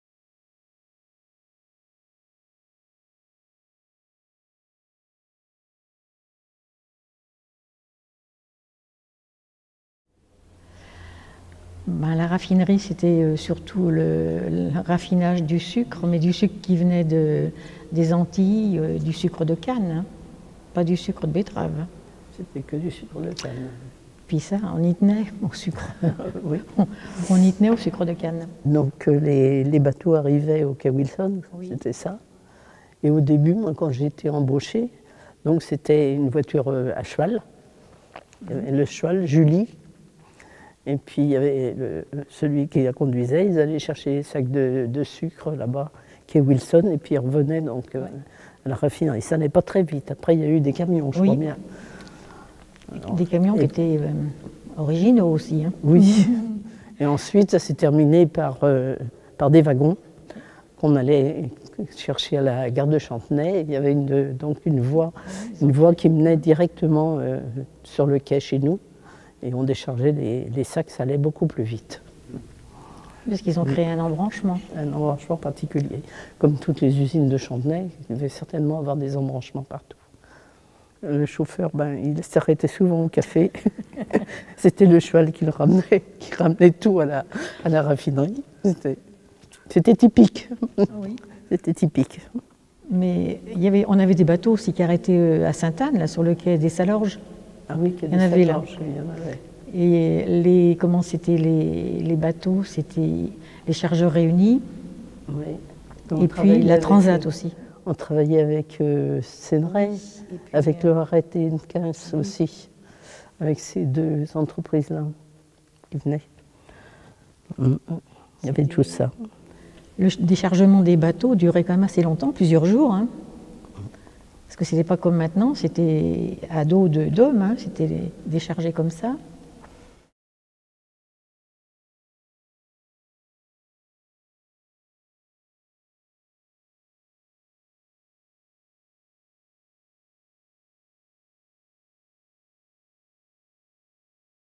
Témoignage : raffineries de sucre et approvisionnement à Chantenay
Mme Juliette Guérinau et Mme Jacqueline Ricordeau, anciennes employées de la Raffinerie de Chantenay, évoquent leur souvenir des différents produits et procédés de production de leur entreprise.

Vidéo réalisée dans le cadre de l'Inventaire du patrimoine culturel du Bas-Chantenay en 2012 en partenariat avec le service patrimoine de la Région Pays de la Loire.